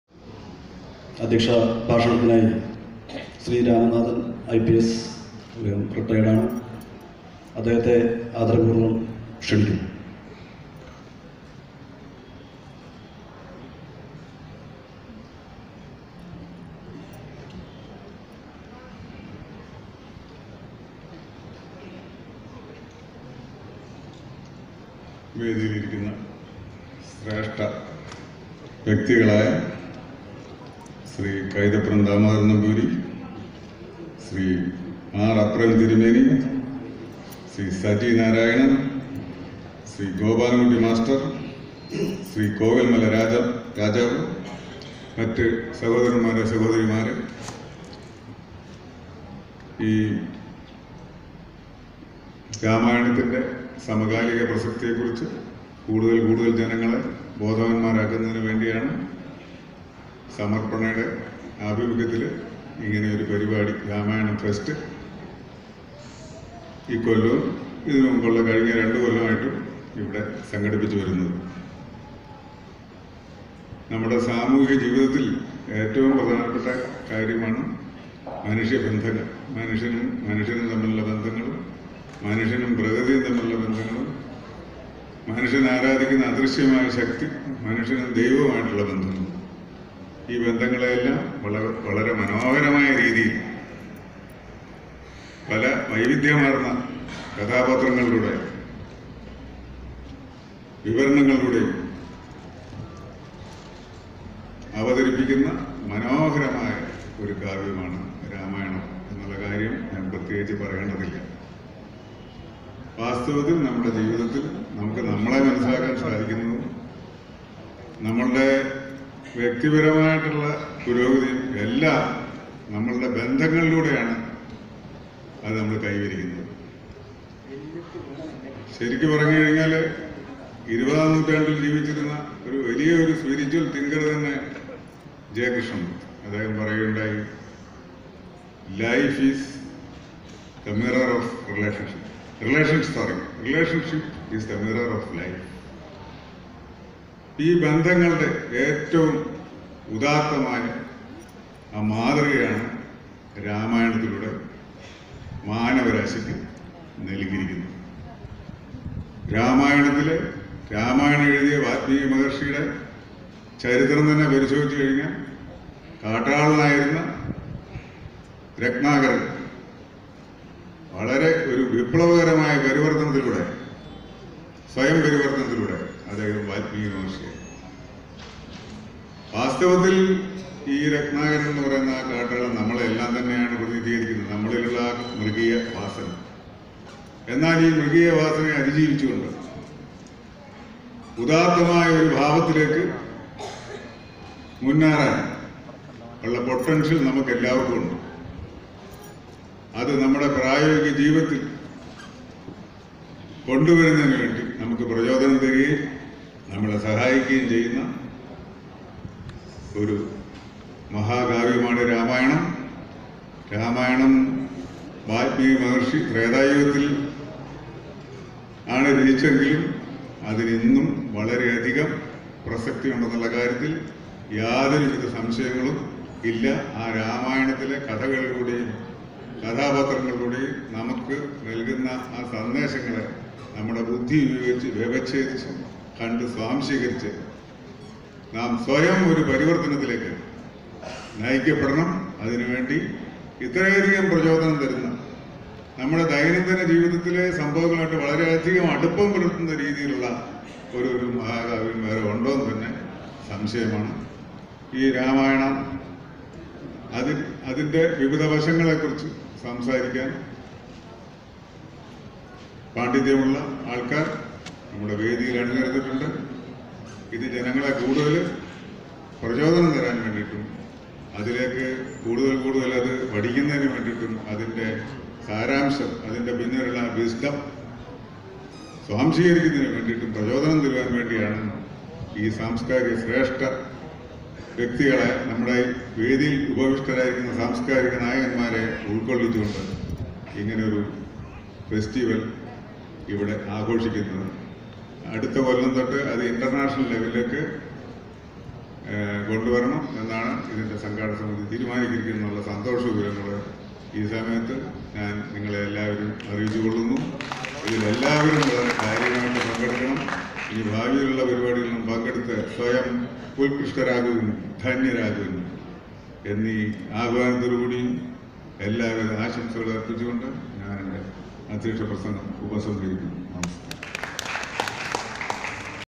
Ramayana Fest / Sabari Fest - 2018. Presidential address
The Ramayana Fest / Sabari Fest - 2018 was celeberated at the Regional Theatre (K.T.Mohammad memorial auditorium) Thrissur by the 'Samarpana' Thrissur.  This is the Presidential address of the concluding session of the programme.